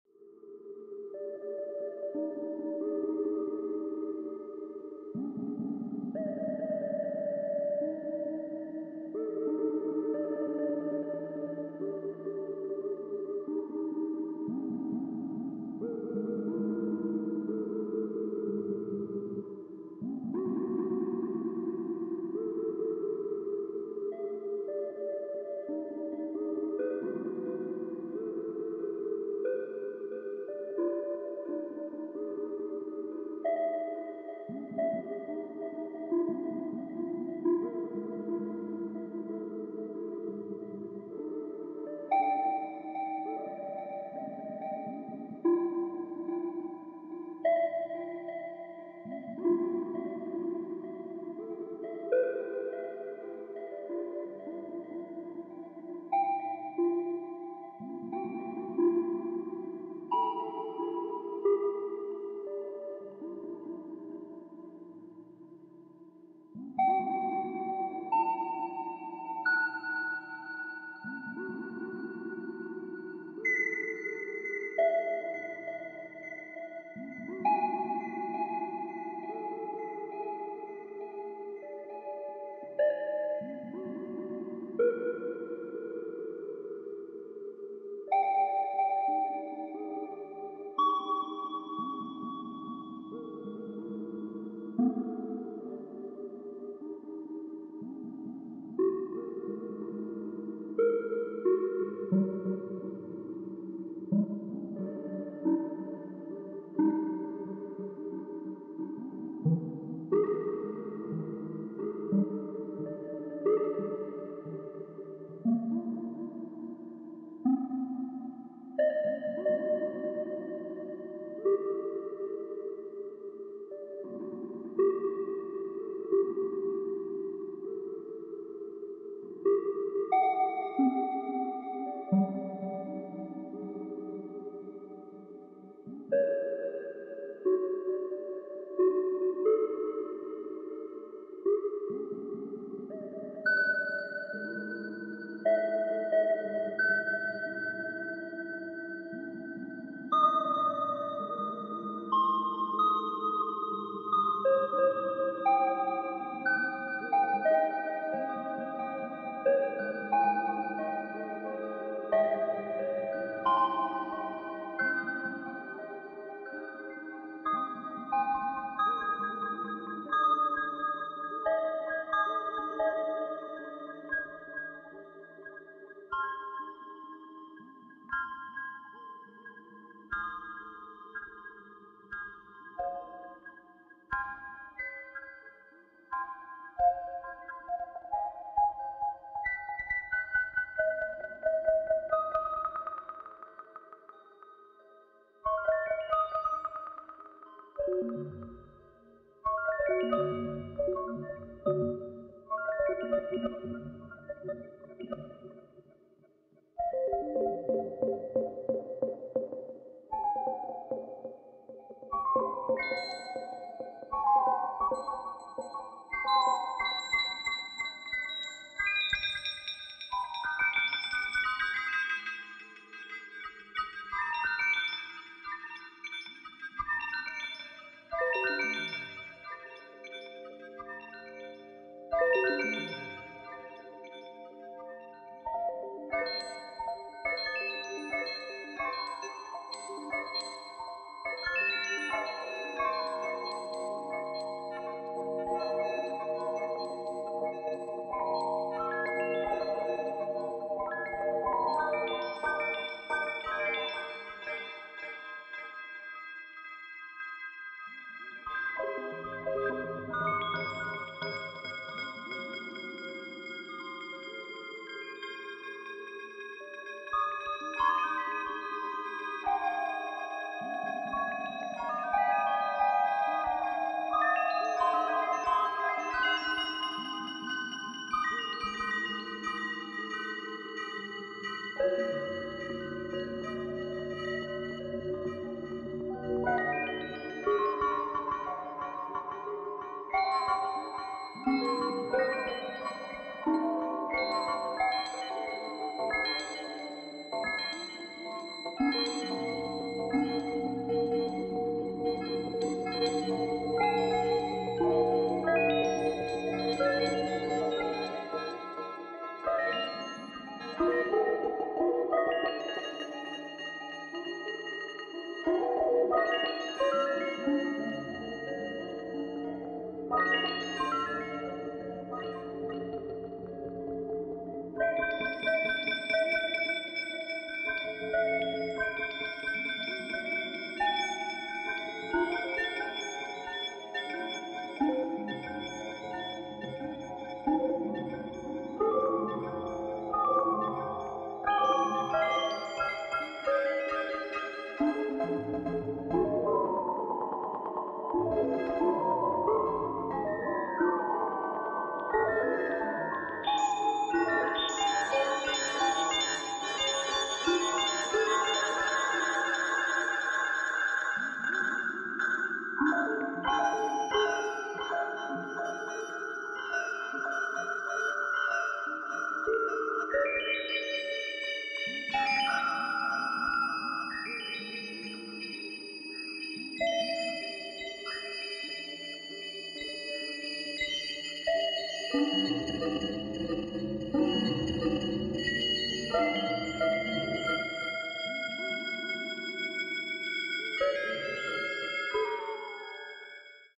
The sound of feedback // Modular patch // Ikarie, Ripples, Melodicer, Mimeophon, Panharmonium, Beads
Playing with feedback is extremely satisfying. In this patch I am pinging Ikarie and Ripples with the Melodicer and processing their output.
There's just something about hearing raw voltage screaming through your speakers that makes it sound alive and organic.
While feedback is usually harsh and unpredictable, it can be beautiful when you treat it nicely.
While most just hear blips and blops, I can get lost for hours and hours in that electrical jungle.